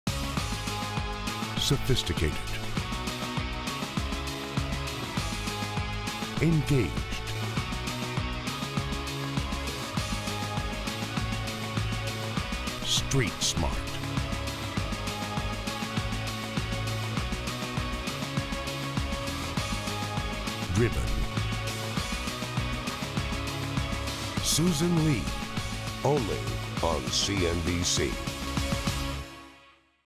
Susan Li - Coming soon to CNBC
Susan Li will make her debut on CNBC next Monday, January 27th.  She will appear across Asia and Europe programming including Squawk Box Asia, The Call, Cash Flow from Australia, Capital Connection and Squawk Box Europe.